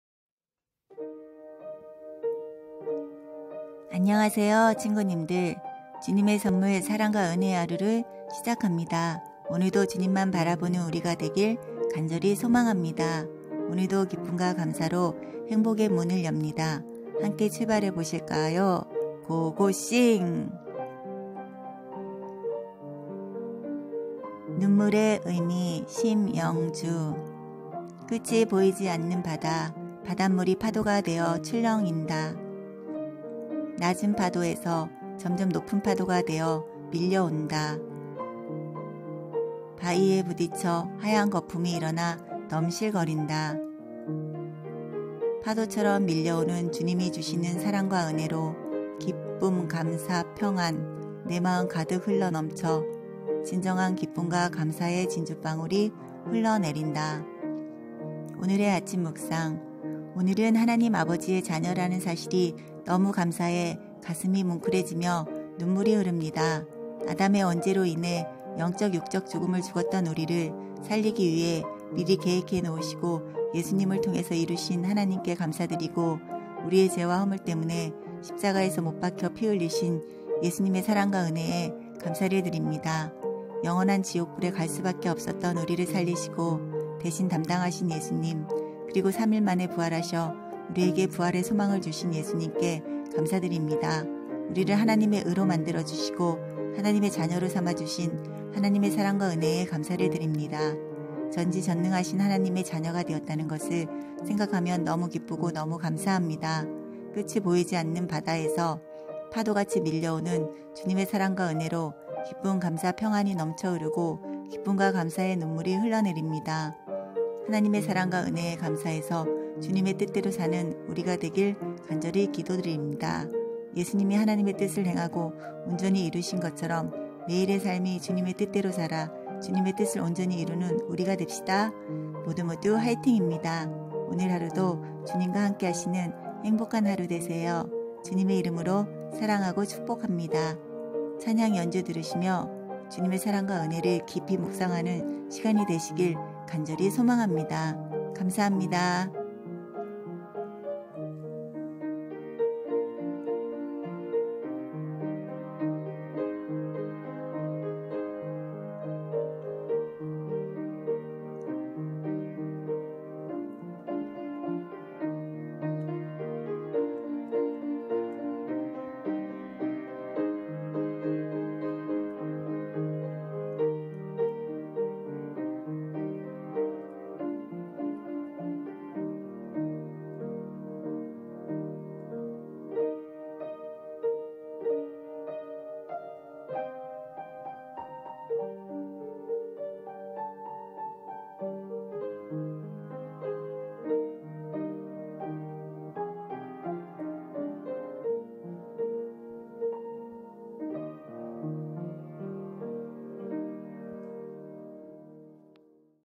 눈물의 의미(성시, 아침묵상)하나님의 마음 심영주
하나님의 자녀라는 것이 너무 기쁘고 감사해 눈물이 흘러내립니다.